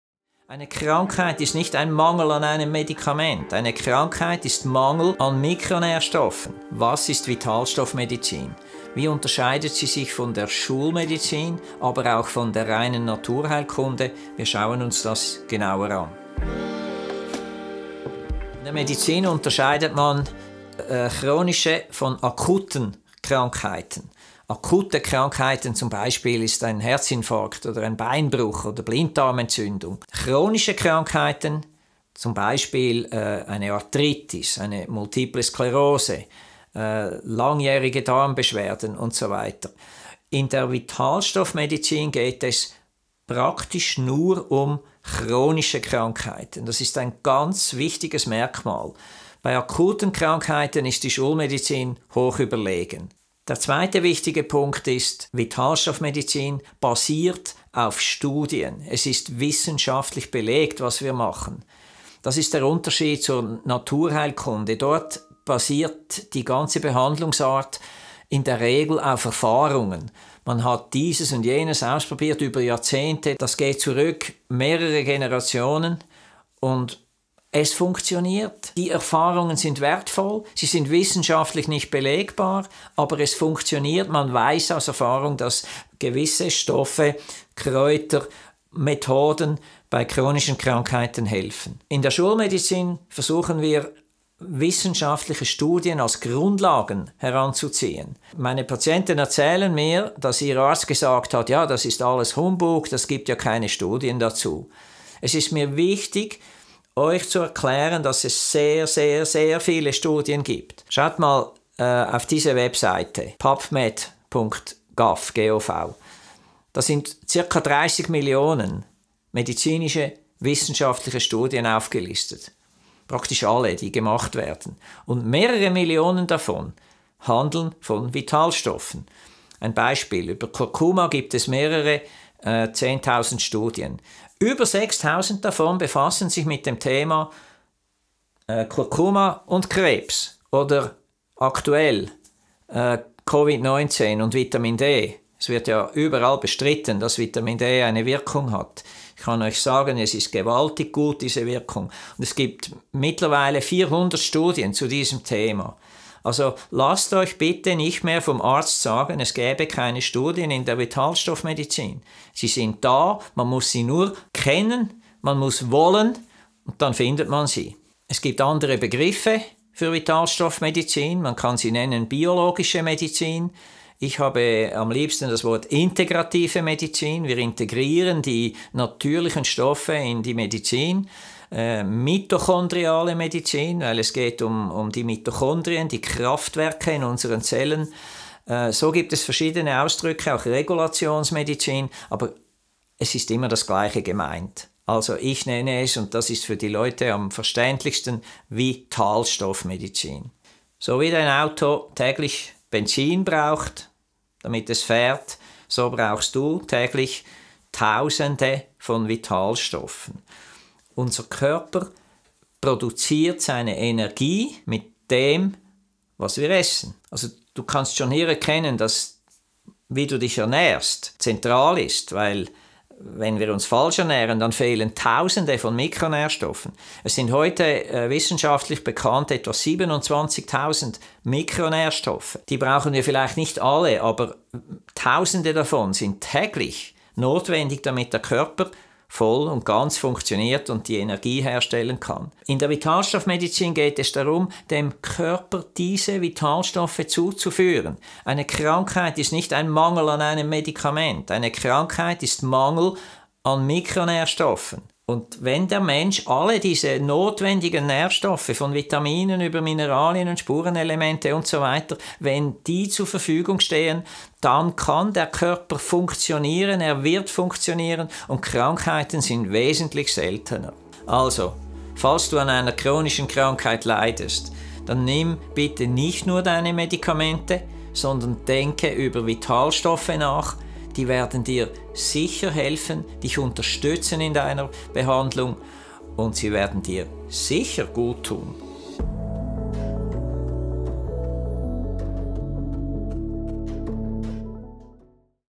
Was ist Vitalstoffmedizin ?
Wie unterscheidet sich die Vitalstoffmedizin von der Schulmedizin, aber auch der Naturheilkunde? Wir schauen uns zudem andere Begriffe an, welche für die Vitalstoffmedizin verwendet werden.
Erfahre mehr über Vitalstoffmedizin, die sich speziell bei chronischen Krankheiten eignet.

0:00 Intro
0:17 Chronische und Akute Krankheiten
1:31 Vitalstoffmedizin und Wissenschaft
2:47 Andere Begriffe der Vitalstoffmedizin
3:20 Wichtigkeit von Vitalstoffe
4:30 Outro

2